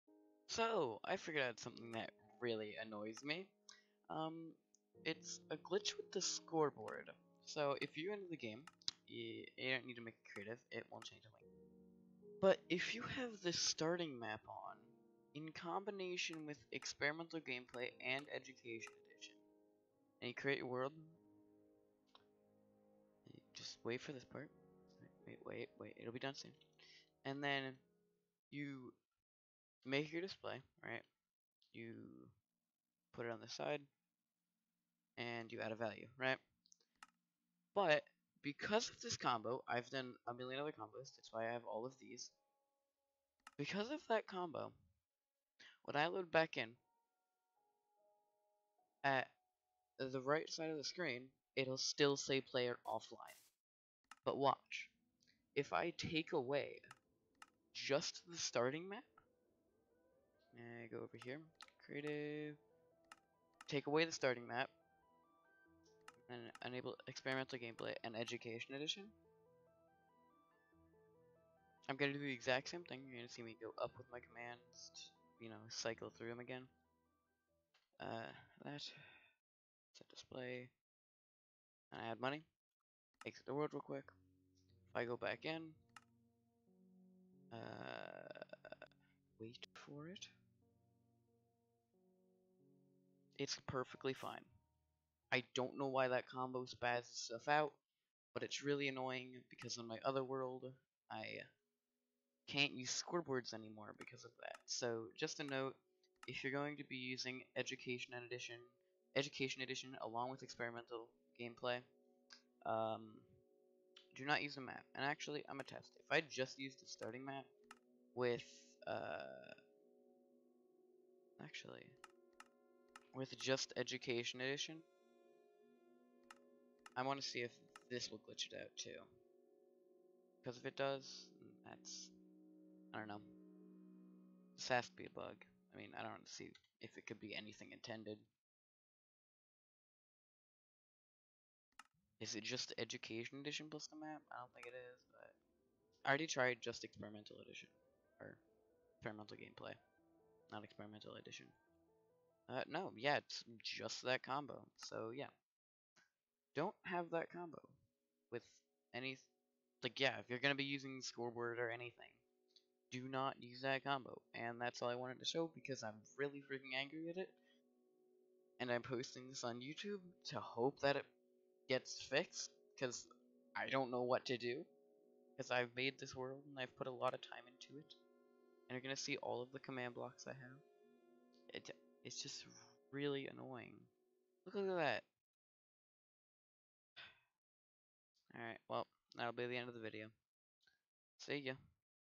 Scoreboard Glitch IS REALLY ANNOYING
if you know a fix plz tell me, no, remaking the scoreboard does nothing, copying the world doesn't do anything either